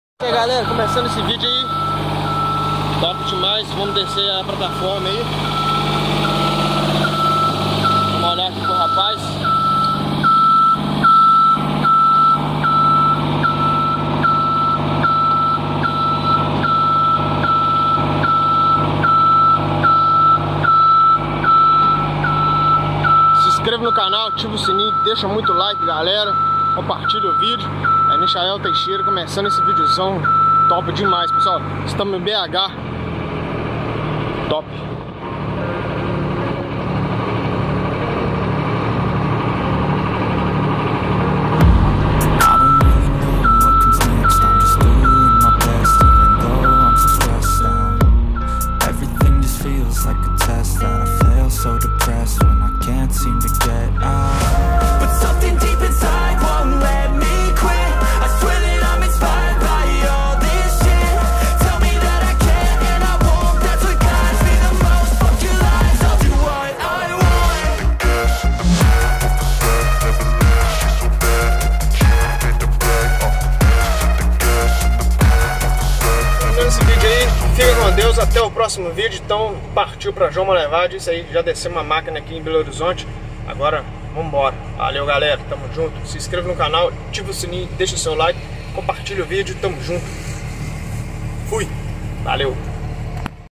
Vamos descarregar está tesoura do caminhão prancha em Belo Horizonte MG
segue no Instagram Michael.teixeira.9231